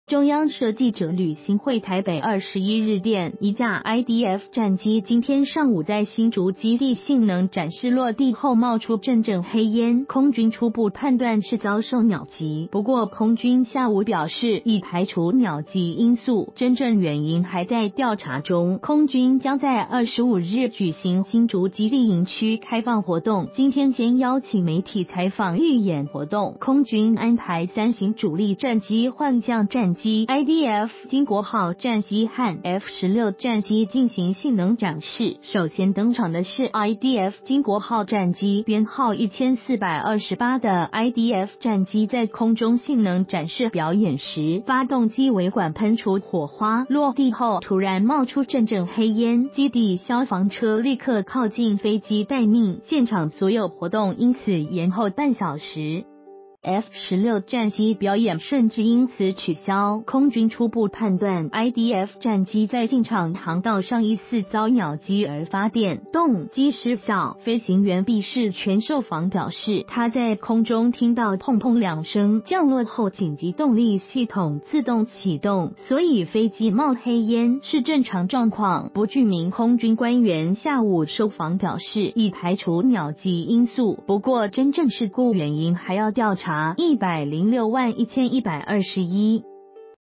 經國號降落後冒黑煙 事故原因排除鳥擊
（中央社記者呂欣憓台北21日電）一架IDF戰機今天上午在新竹基地性能展示落地後冒出陣陣黑煙。空軍初步判斷是遭受鳥擊，不過空軍下午表示，已排除鳥擊因素，真正原因還在調查中。空軍將在25日舉行新竹基地營區開放活動，今天先邀請媒體採訪預演活動，空軍安排三型主力戰機幻象戰機、IDF經國號戰機和F-16戰機進行性能展示。首先登場的是IDF經國號戰機，編號1428的IDF戰機在空中性能展示表演時，發動機尾管噴出火花，落地後突然冒出陣陣黑煙，基地消防車立刻靠近飛機待命，現場所有活動因此延後半小時，F-16戰機表演甚至因此取消。空軍初步判斷IDF戰機在進場航道上疑似遭鳥擊而發電（動）機失效，飛行員畢世全受訪表示，他在空中聽到碰碰兩聲，降落後緊急動力系統自動啟動，所以飛機冒黑煙，是正常狀況。不具名空軍官員下午受訪表示，已排除鳥擊因素，不過真正事故原因還要調查。1061121

發表評論評論如下。